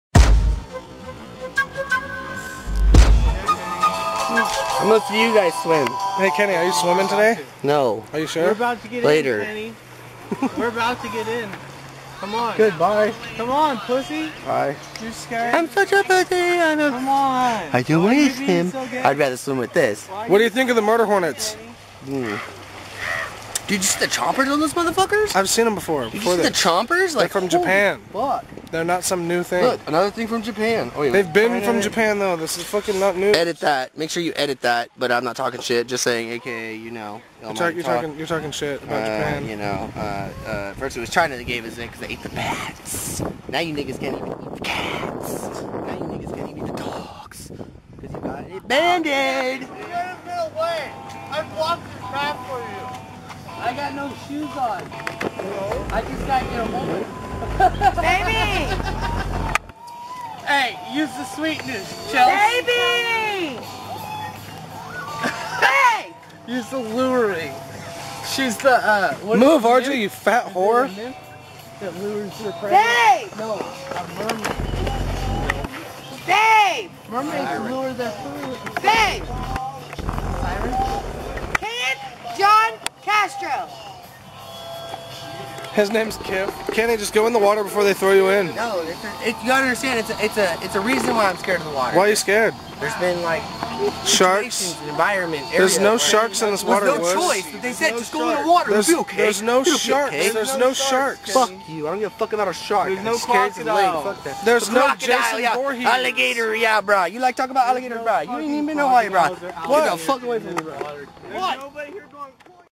Kenny Can't swim
Kenny doesn't trust that there aren't sharks in camp far west lake. Of all the dumb shit to think haha he takes the tard cake.